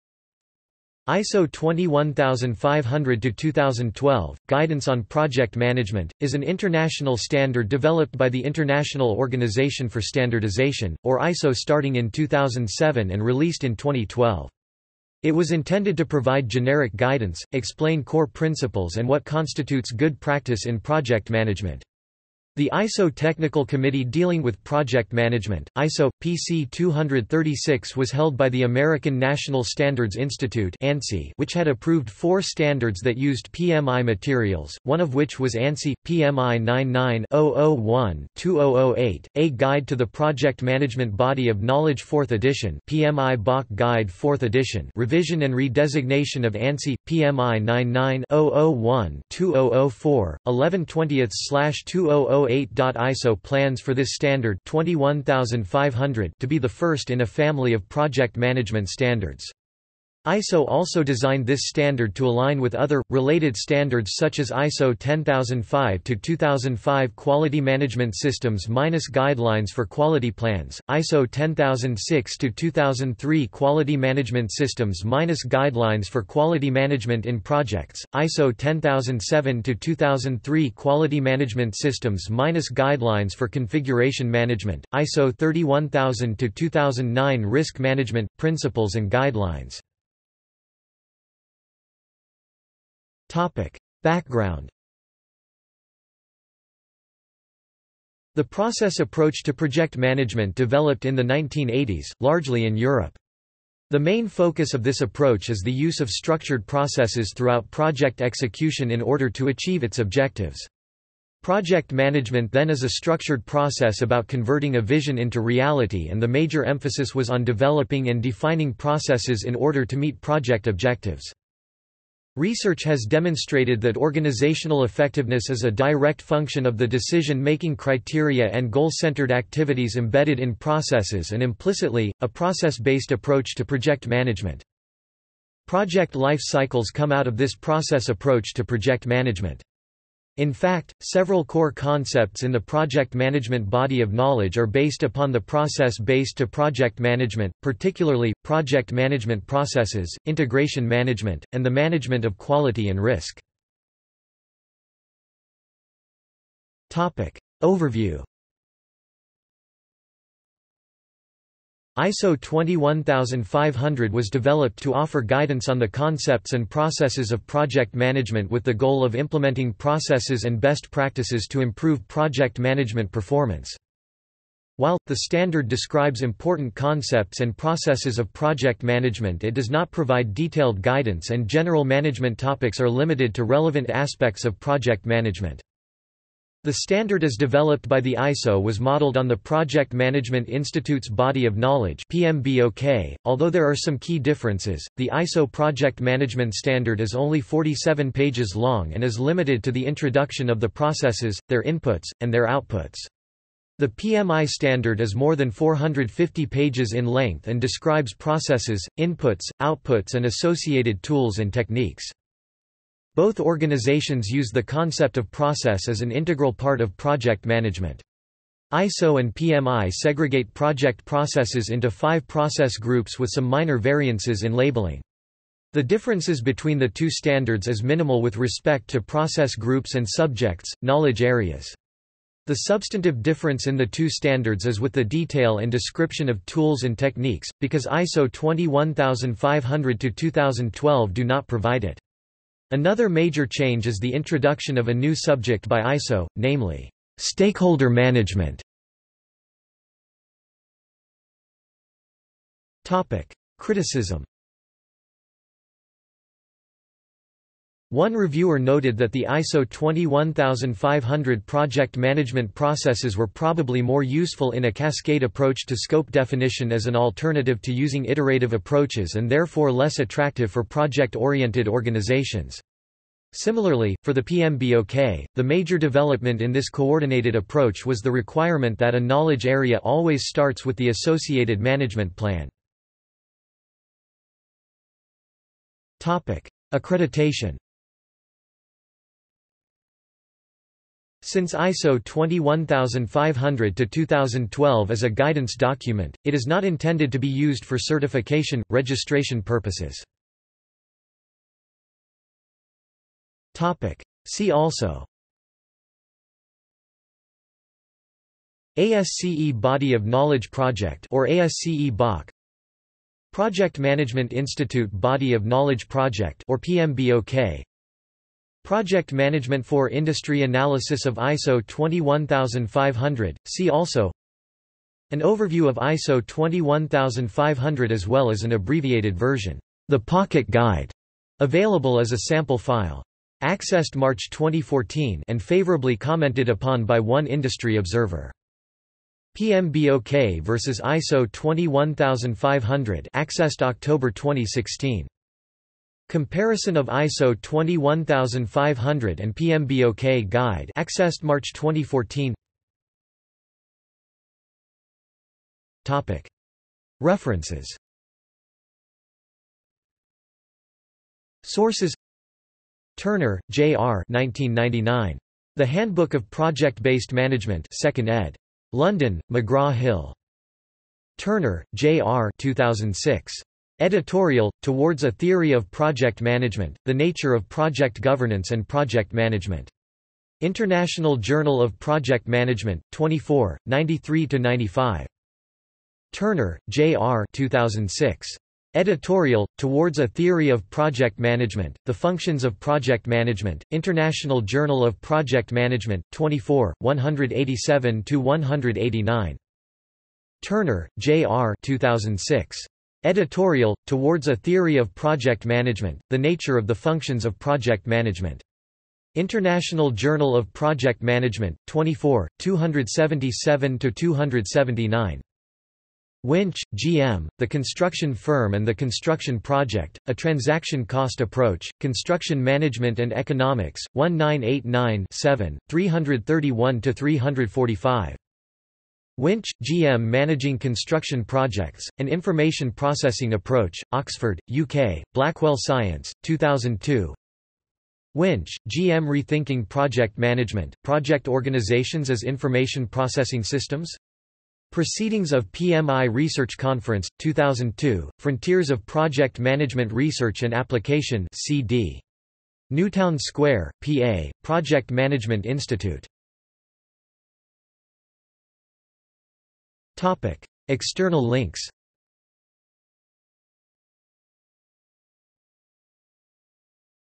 ISO 21500 | Wikipedia audio article
This is an audio version of the Wikipedia Article:
ISO 21500

- Socrates

SUMMARY
ISO 21500:2012, Guidance on Project Management, is an international standard developed by the International Organization for Standardization, or ISO starting in 2007 and released in 2012. It was intended to provide generic guidance, explain core principles and what constitutes good practice in project management. The ISO technical committee dealing with project management, ISO/PC 236 was held by the American National Standards Institute (ANSI) which had approved four standards that used PMI materials. one of which was ANSI/PMI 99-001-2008, A Guide to the Project Management Body of Knowledge - 4th Edition (PMI BoK® Guide - 4th Edition) (revision and re-designation of ANSI/PMI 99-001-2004): 11/20/2008.ISO plans for this standard (21500) to be the first in a family of project management standards. ISO also designed this standard to align with other, related standards such as ISO 10005:2005 Quality management systems − Guidelines for quality plans, ISO 10006:2003 Quality management systems − Guidelines for quality management in projects, ISO 10007:2003 Quality management systems − Guidelines for configuration management, ISO 31000:2009 Risk management – Principles and guidelines.